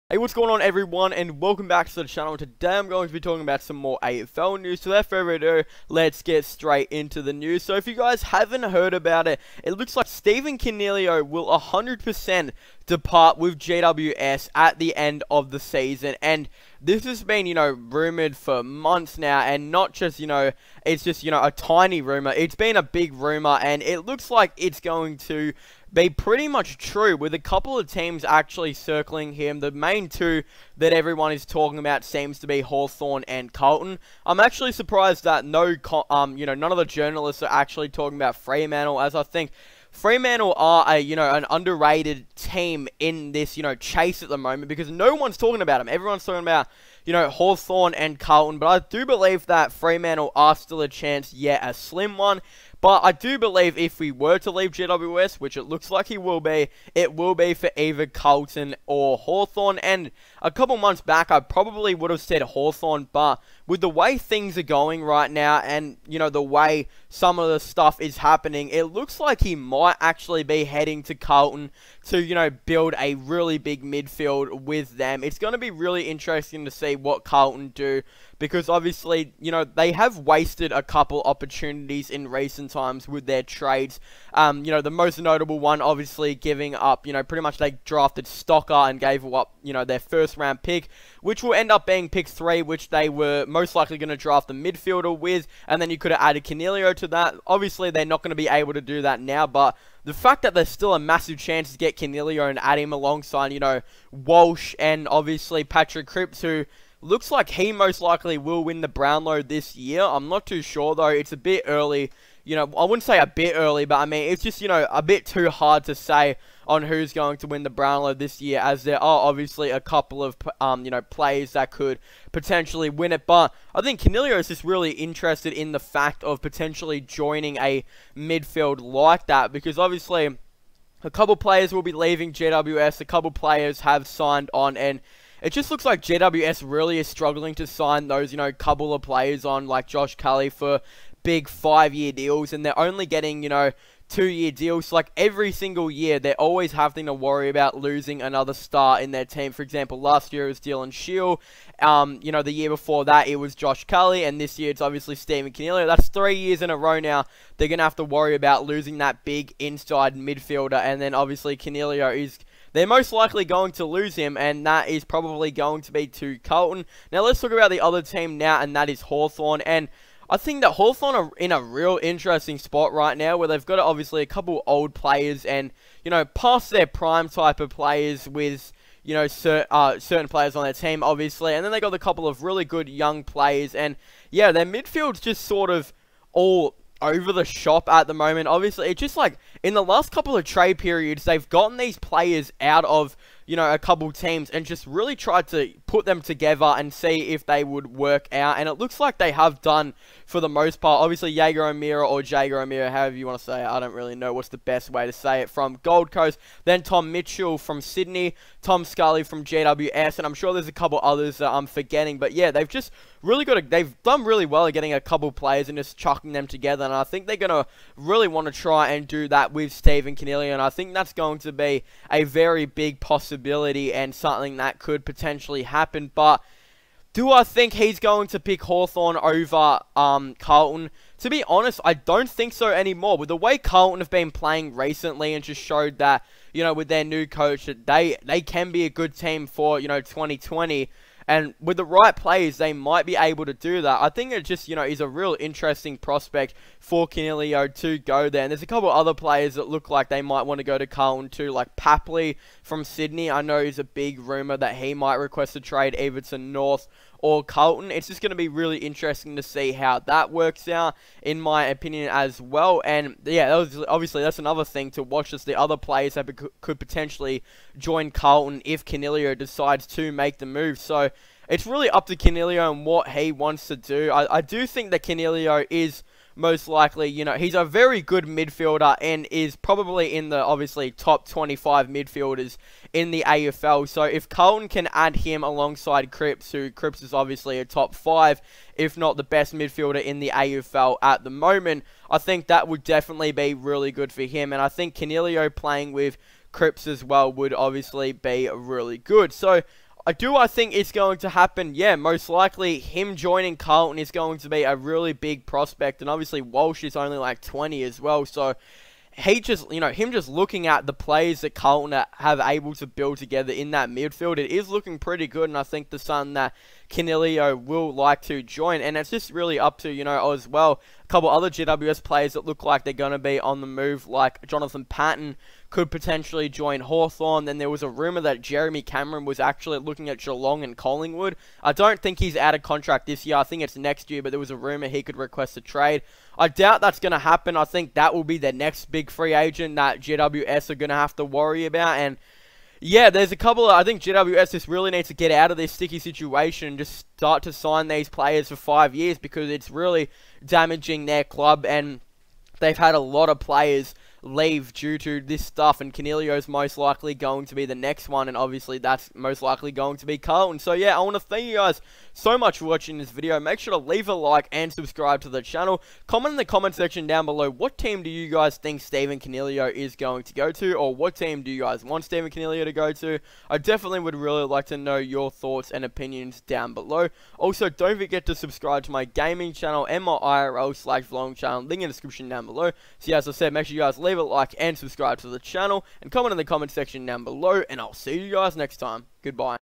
Stephen Coniglio To Leave GWS!!! Hawthorn, Carlton & More Rumored!!!
Don't forget to tell me what team you think Stephen Coniglio should go to in the comments!!!